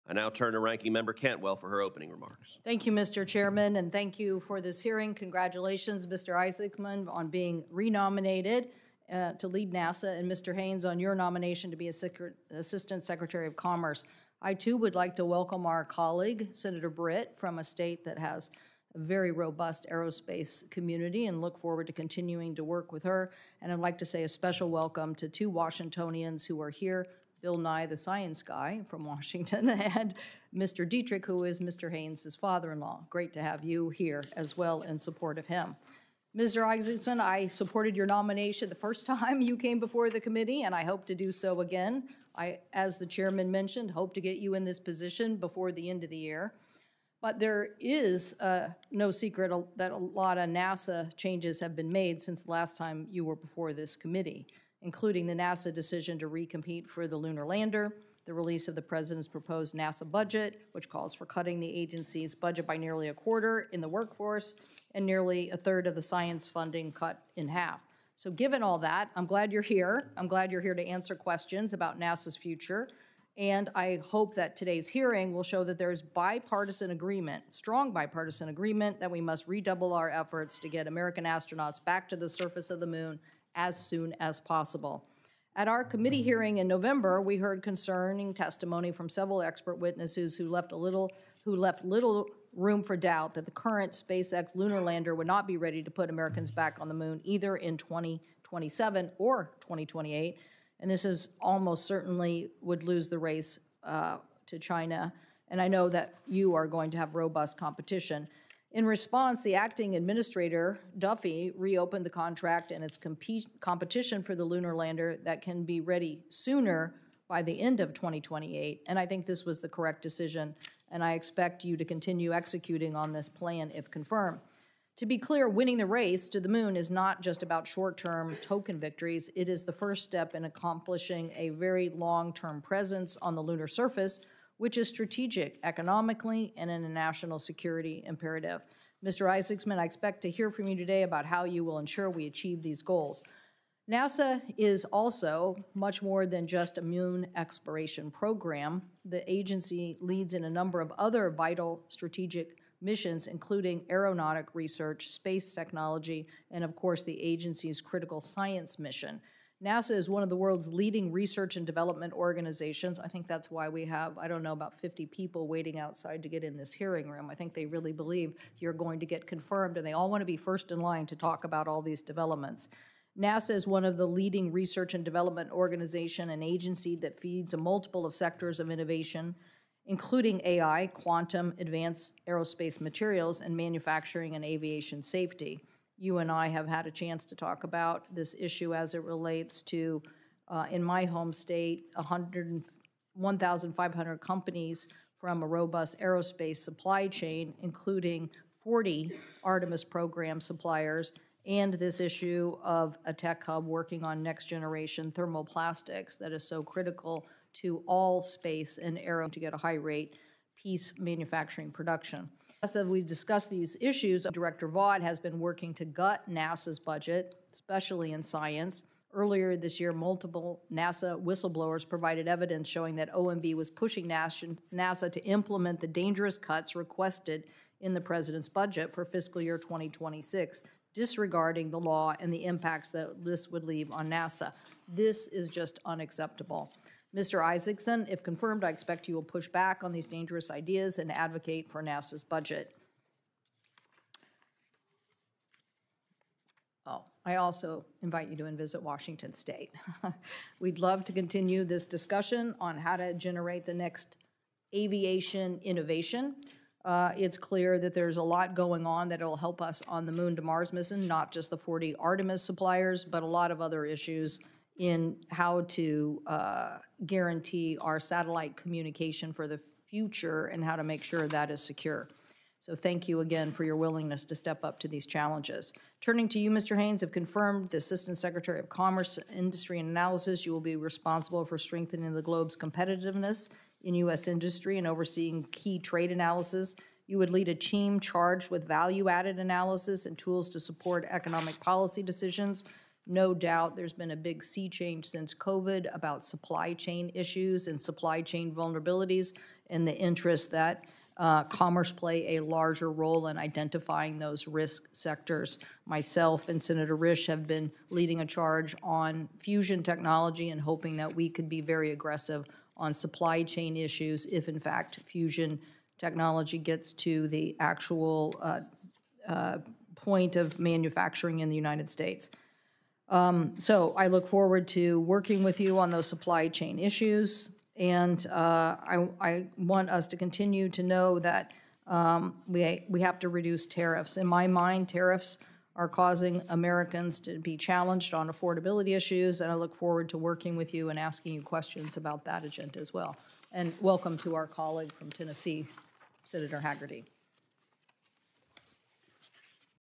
'This Is Just Unacceptable': Maria Cantwell Lambasts Proposed Funding Cuts To NASA Research
During a Senate Commerce Committee hearing earlier this month, Sen. Maria Cantwell (D-WA) spoke about funding for NASA.